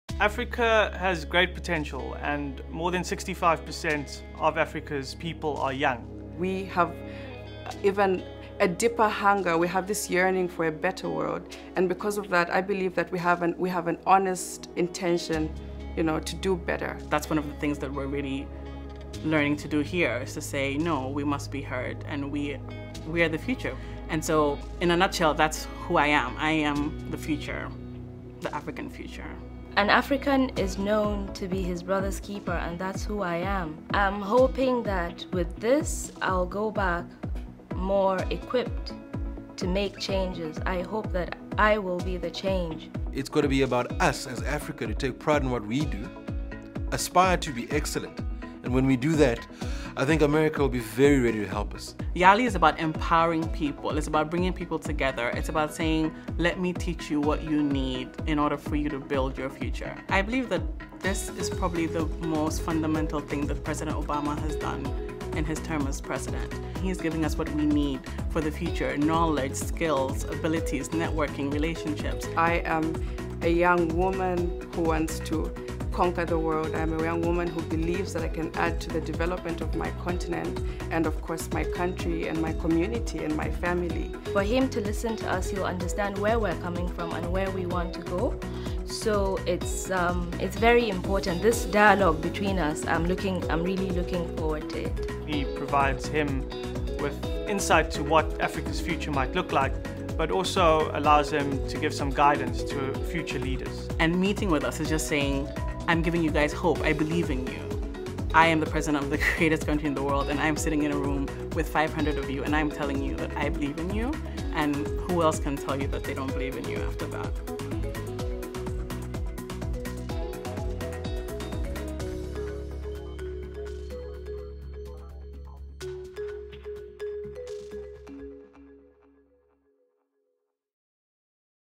The Young African Leaders Initiative
The Obama administration’s Young African Leaders Initiative (YALI) is a signature effort to invest in the next generation of African leaders. Nearly 1 in 3 Africans are between the ages of 10 and 24, and approximately 60 percent of Africa’s total population is below the age of 35. President Obama launched YALI in 2010 to support young African leaders as they spur growth and prosperity, strengthen democratic governance, and enhance peace and security across Africa.